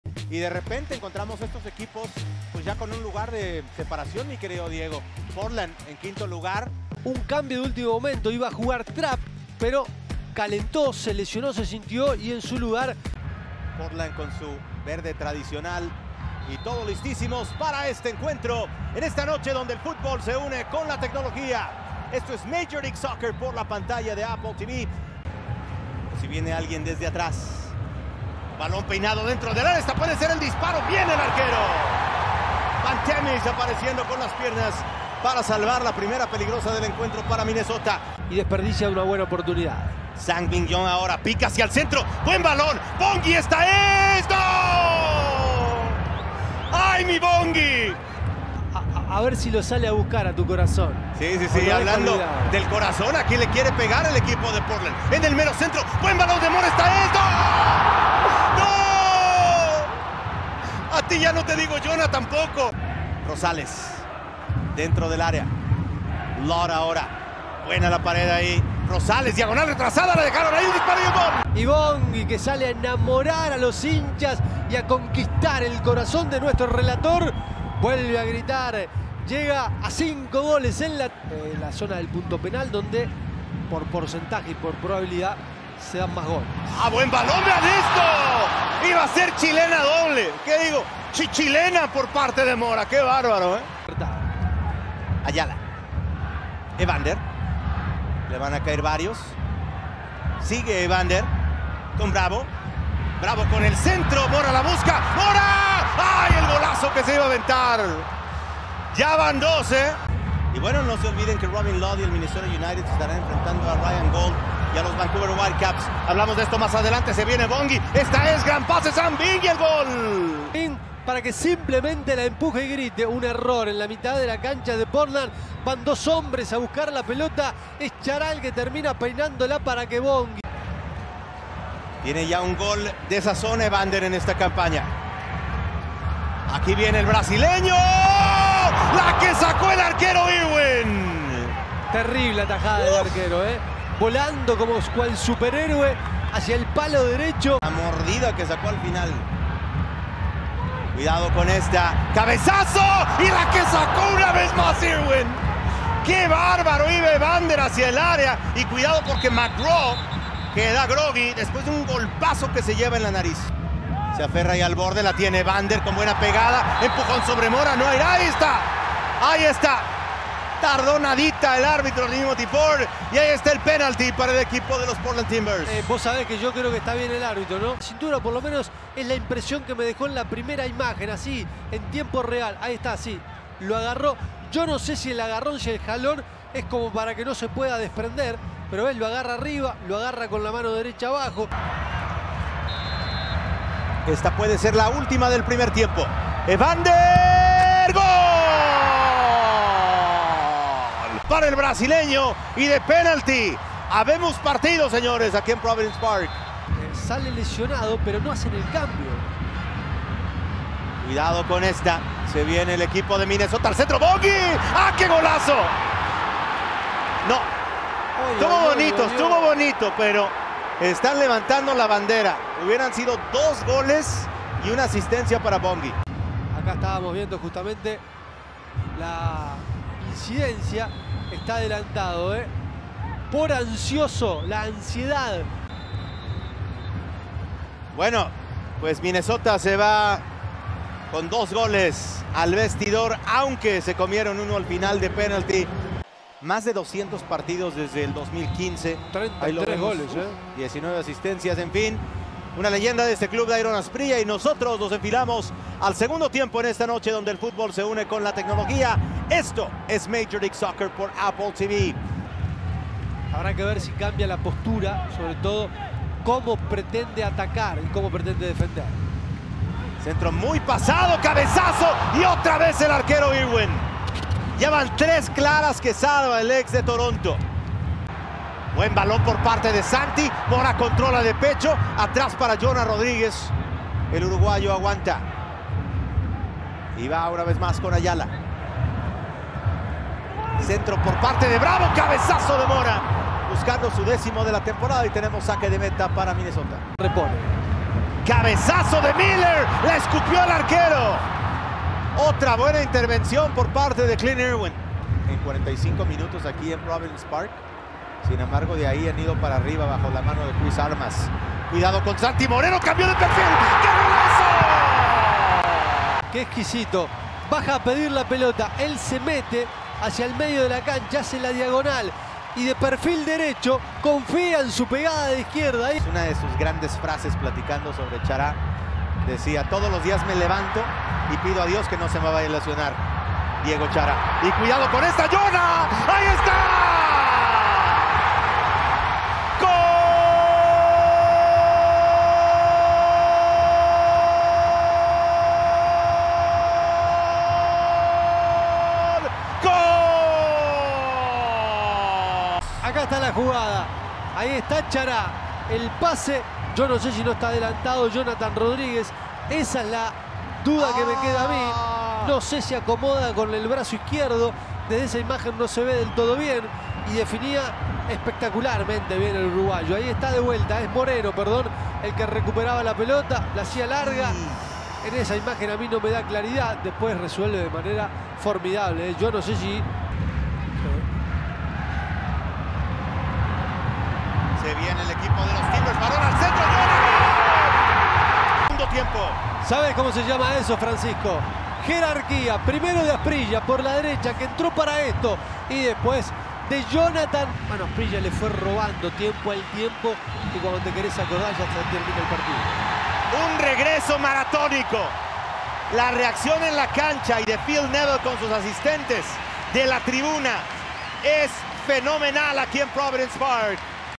RESUMEN | ¡Asprilla al rescate! Timbers le dan vuelta al partido, ganan 3-2 | 29 de junio del 2024
Los Portland Timbers derrotaron a Minnesota United FC por 3-2 en Providence Park. Tras ir perdiendo 0-2 al minuto 38, Portland volteo el encuentro con goles de Evander (penal), Santiago Moreno, y Jonathan Rodríguez (asistencia de Asprilla). Partido correspondiente a la jornada 21 de Portland en MLS.

Sigue toda la acción en vivo en *MLS Season Pass* por  Apple TV app